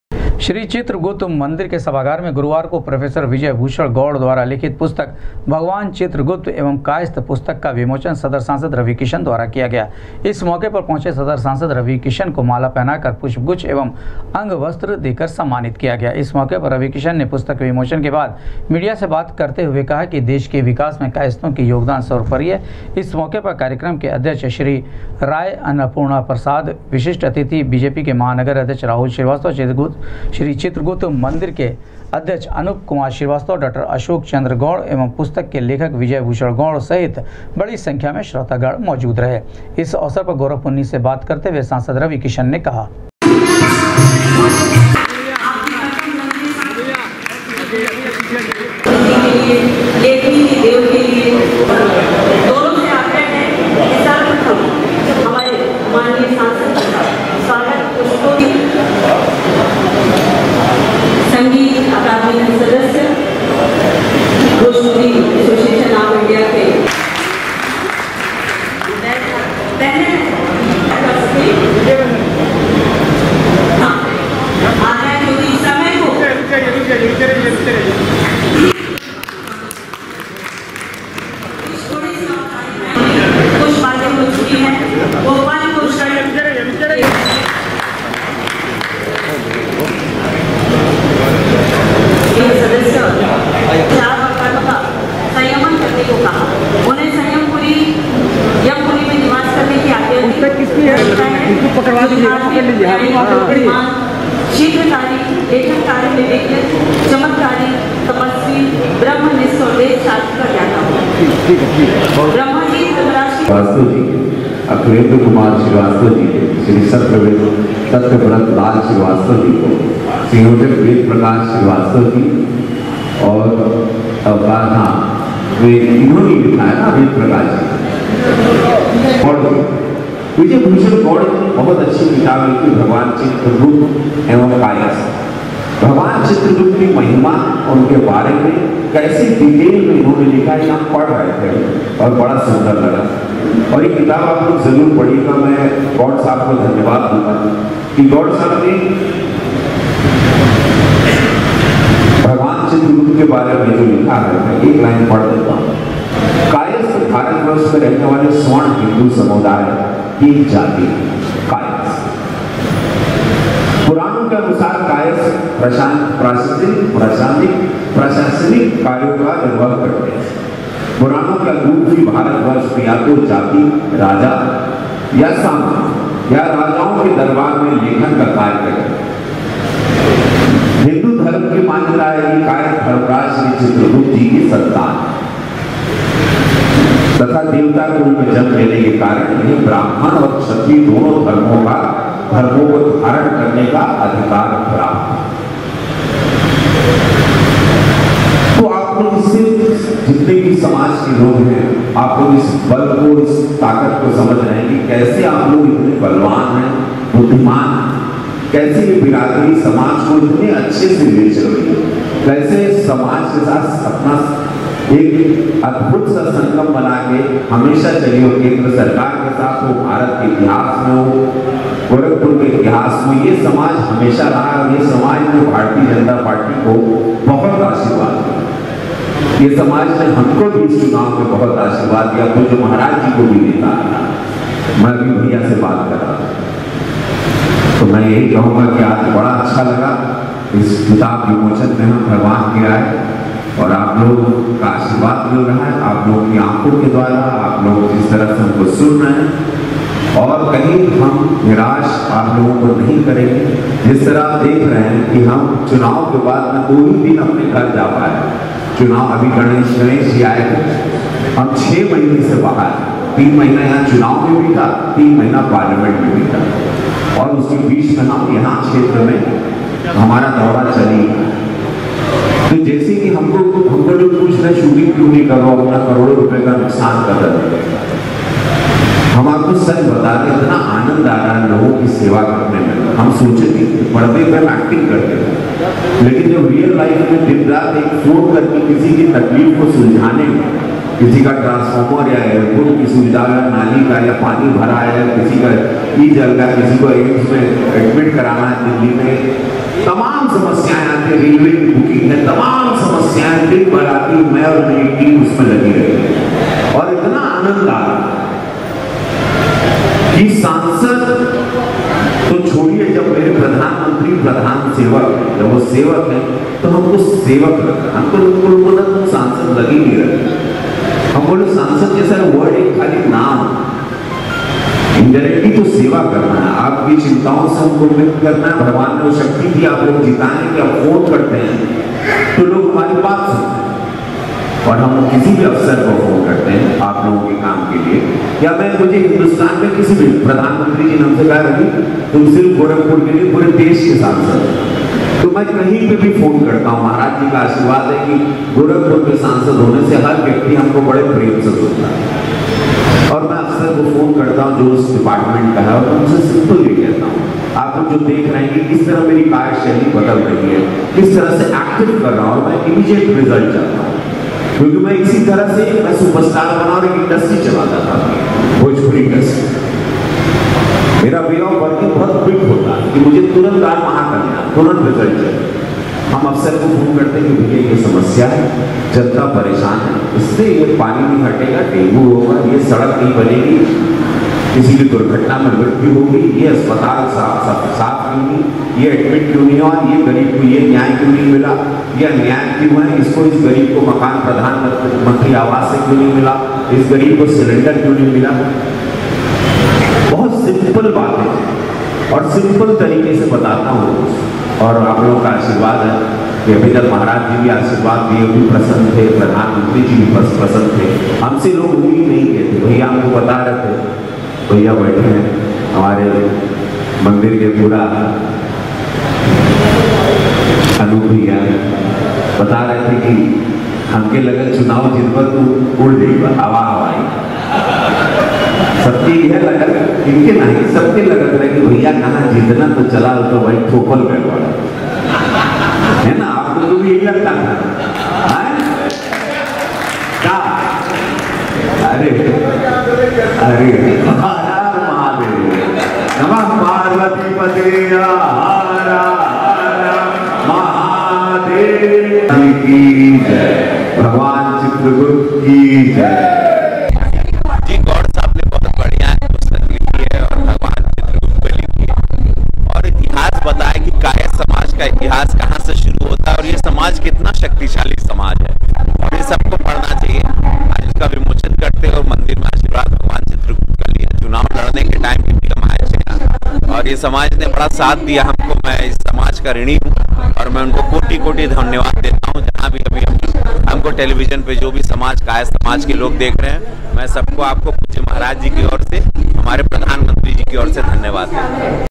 प्रो0 विजय भूषण गौड़ द्वारा लिखित पुस्तक ‘भगवान चित्रगुप्त एवं कायस्थ’ का सदर सांसद रवि किशन ने
प्रो0 विजय भूषण गौड़ द्वारा लिखित पुस्तक ‘भगवान चित्रगुप्त एवं कायस्थ’ का सदर सांसद रवि किशन ने किया विमोचन,कहा देश के विकास में कायस्थों का योगदान है सर्वोपरि